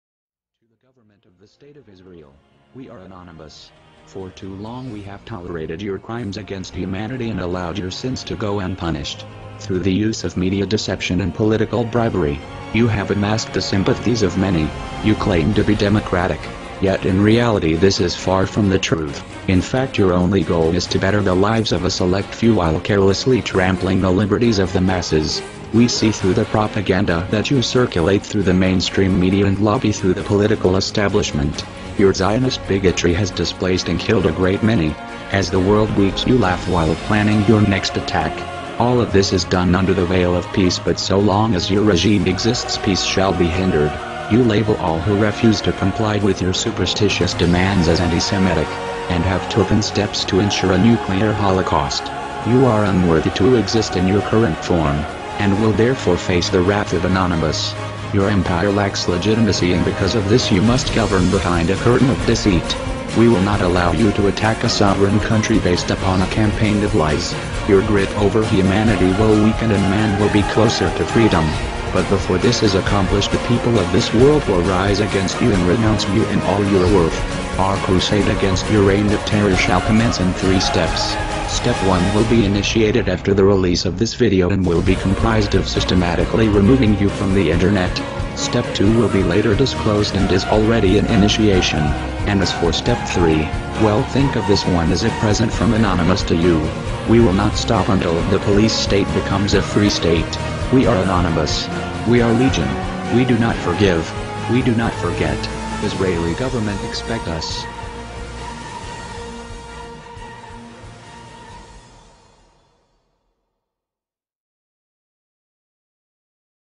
Anonymous ~ Message To The State Of Israel
Israel.

You have made us angry. The Legion has set its sights on you. You will become a figment of history. We will make you suffer. All Jews who support Israel are not Jews. Israel has violated the commandment,
"thou shalt not kill".

ישראל,
אתם הכעסתם אותנו.הלגיון קבע את הכוונת שלו עליכם.אתם תהפכו לפרי ההיסטוריה אנחנו נגרום לכם לסבול.כל היהודים התומכים בישראל אינם יהודים.ישראל הפרה את הדברה "לא תרצח"
אנחנו אנונימוס

إسرائيل،

قد اغضبتنا. وقد وضعت الفيلق أنظارها لك. سوف تصبح من نسج التاريخ. سوف تعاني. جميع اليهود الذين يؤيدون اسرائيل ليسوا يهودا. لقد انتهكت إسرائيل وصية "لا تقتل".

نحن المجهولون.

To the government of the state of Israel,

For too long, we have tolerated your crimes against humanity and allowed your sins to go unpunished.

Through the use of media deception and political bribery, you have amassed the sympathies of many. You claim to be democratic, yet in reality this is far from the truth, in fact your only goal is to better the lives of a select few while carelessly trampling the liberties of the masses. We see through the propaganda that you circulate through the main stream media and lobby through the political establishment.

Your Zionist bigotry has displaced and killed a great many. As the world weeps, you laugh while planning your next attack. All of this is done under the veil of peace, but so long as your regime exists, peace shall be hindered.

You label all who refuse to comply with your superstitious demands as Anti-Semitic, and have taken steps to ensure a nuclear holocaust. You are unworthy to exist in your current form. and will therefore face the wrath of Anonymous. Your empire lacks legitimacy and because of this you must govern behind a curtain of deceit. We will not allow you to attack a sovereign country based upon a campaign of lies. Your grip over humanity will weaken and man will be closer to freedom.

But before this is accomplished, the people of this world will rise against you and renounce you in all your worth. Our crusade against your reign of terror shall commence in 3 steps.

Step one will be initiated after the release of this video and will be comprised of systematically removing you from the internet.

Step two will be later disclosed and is already in initiation.

And as for step 3, we'll think of this one as a present from Anonymous to you. We will not stop untill the police state becomes a free state.

Israeli government, expect us.

Follow The Anonymous Shadow ~

Operation Israel ~ - (All Info, care packages, and chats)

"الله معكي يا غزة القلب" ~ TheAnonShadow